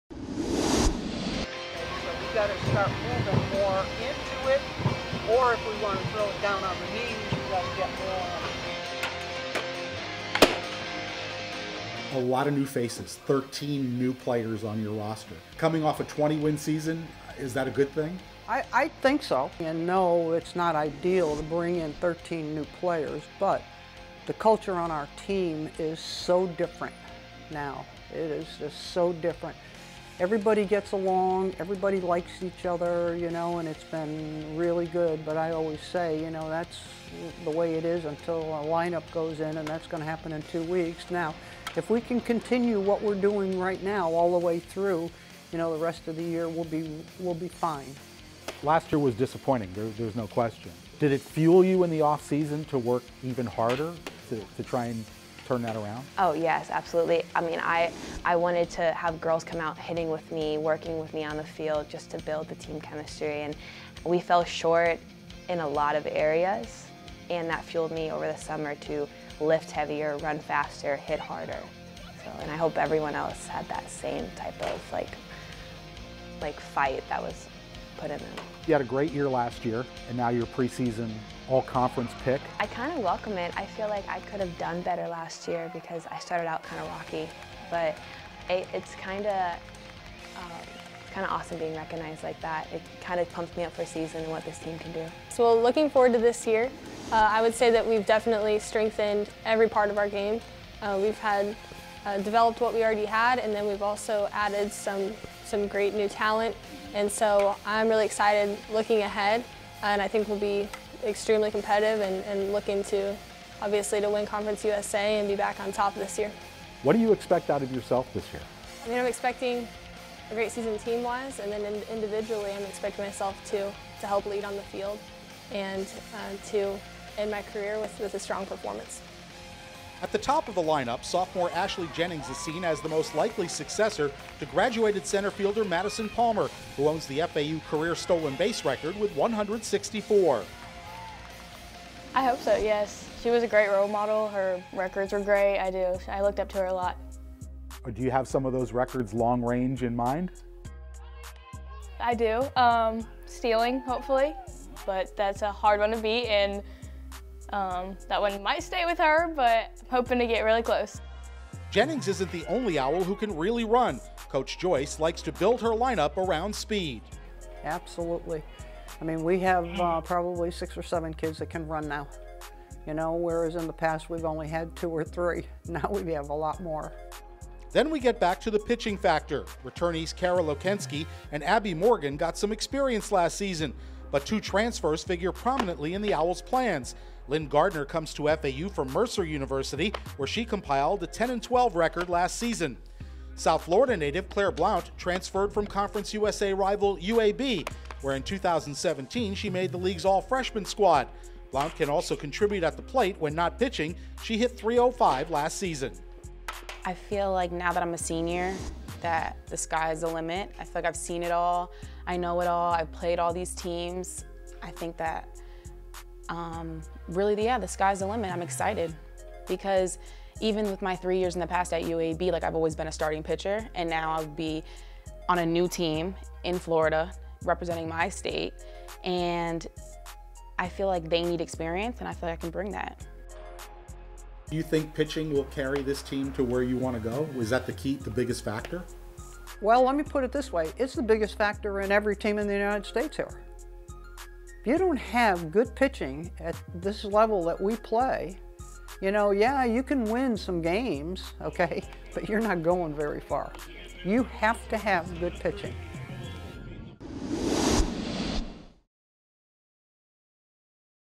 FAU SOFTBALL PREVIEW 2020
The 2020 FAU softball season commences Friday at 6 p.m. against Wisconsin as part of the annual FAU First Pitch Classic.